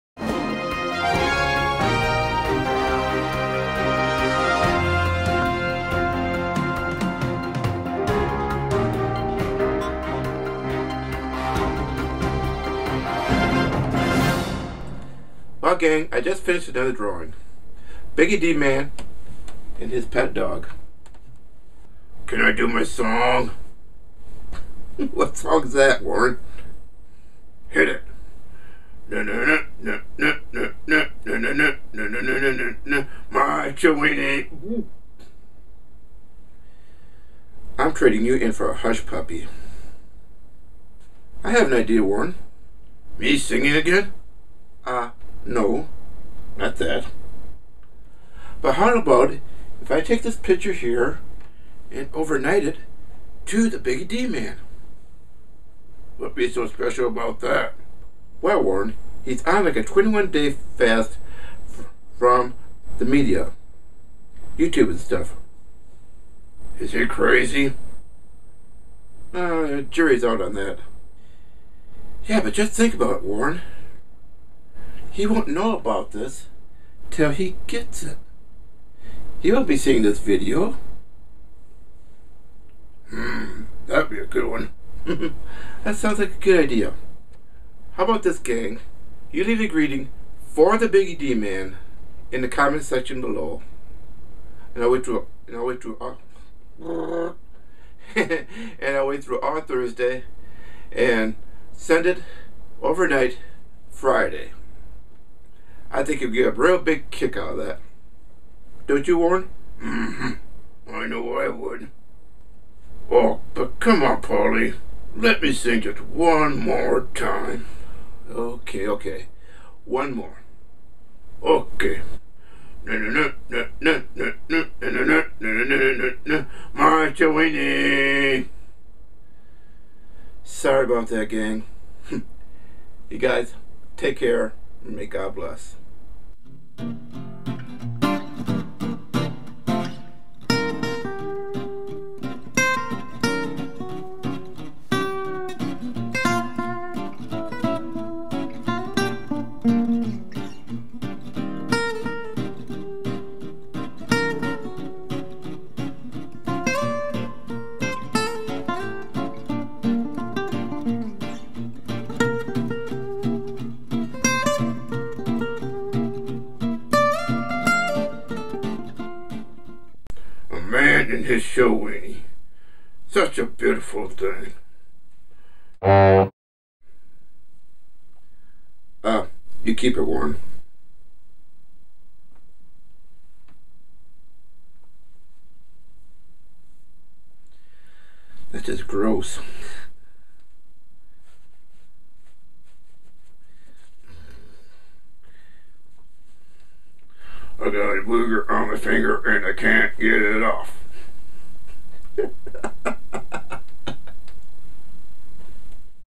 BiggyDman and His Chiweenie
A finished drawing of biggydman  and his Chiweenie dog.
Please leave a greeting to my friend, biggydman down below and I'll pront off the page and mail it to him with the drawing. Thanks. ((Smile))